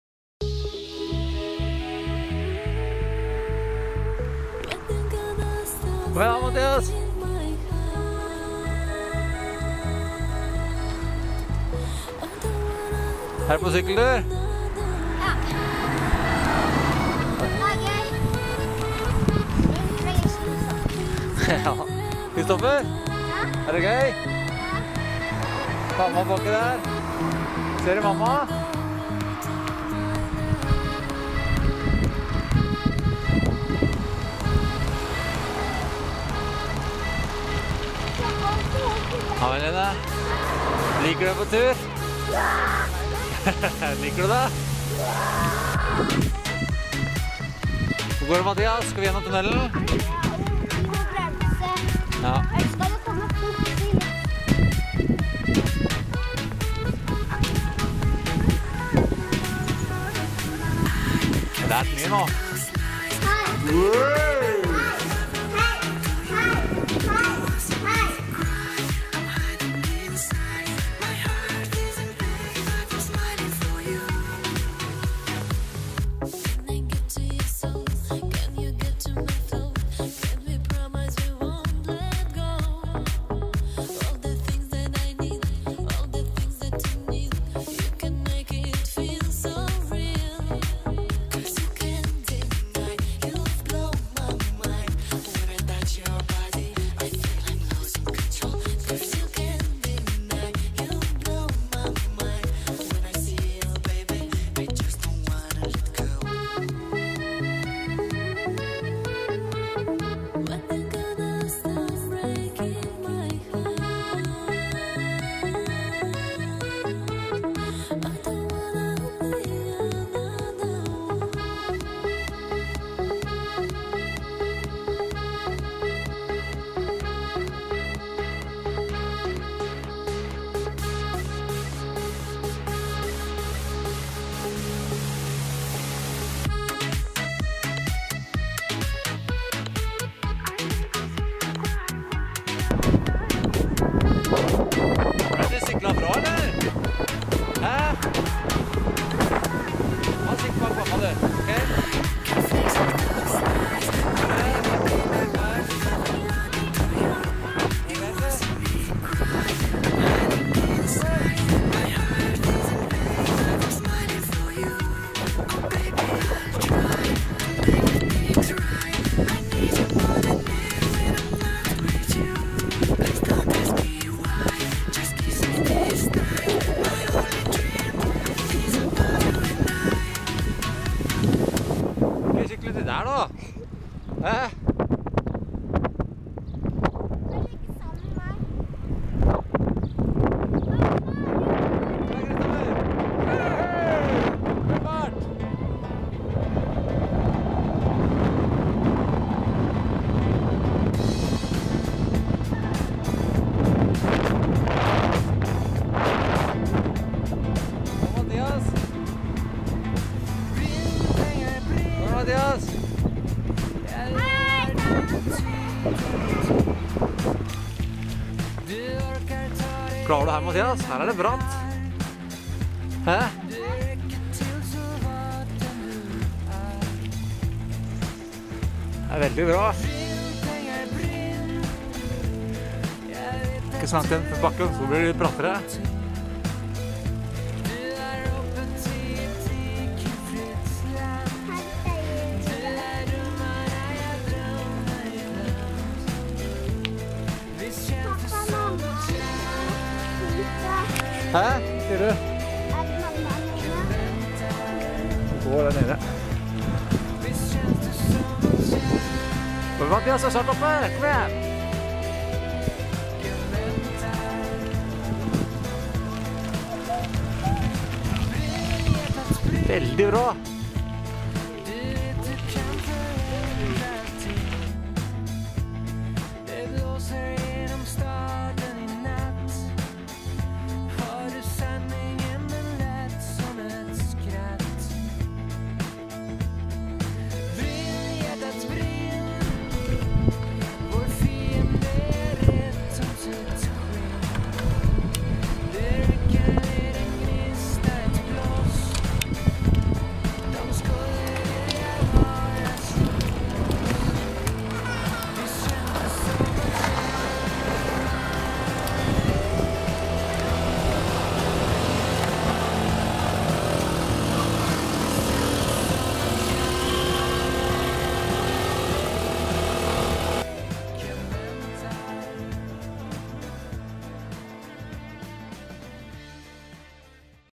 Sykkeltur 04.07.2010
Sykkeltur til Langesund med familien